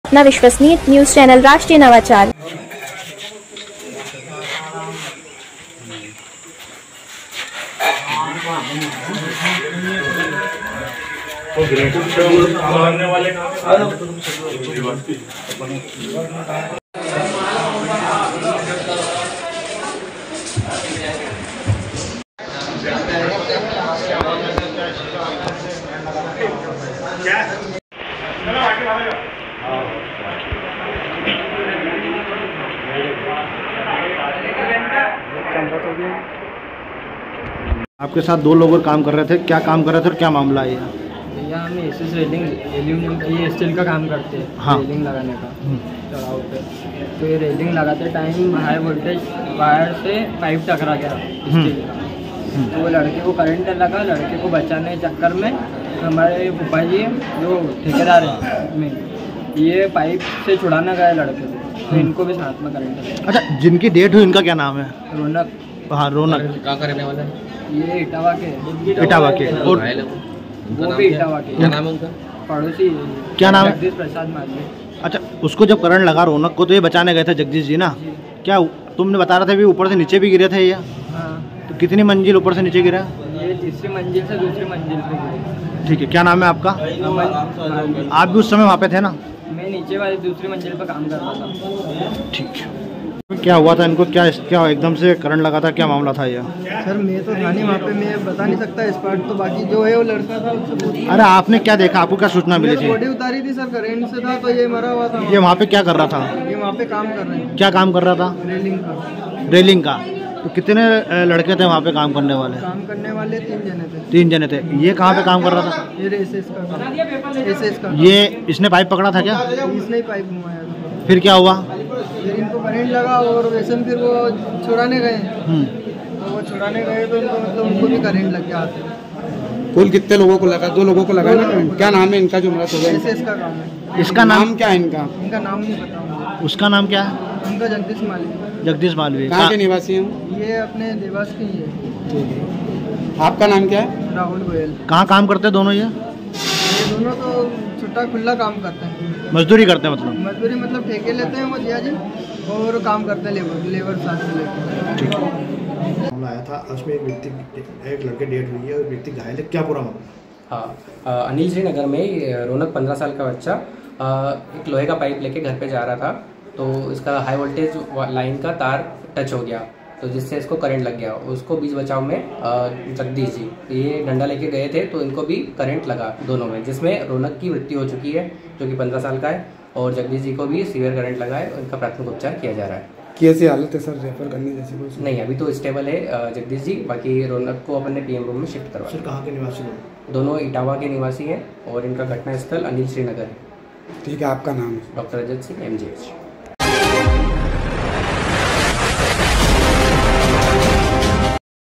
अनिलश्री नगर में रैलिंग लगाने के दौरान करंट से युवक की मौत, बचाने गया दूसरा व्यक्ति भी आया चपेट में
रैलिंग लगाने के दौरान करंट लगने से युवक की मौत, बचाने गया दूसरा व्यक्ति भी आया चपेट में।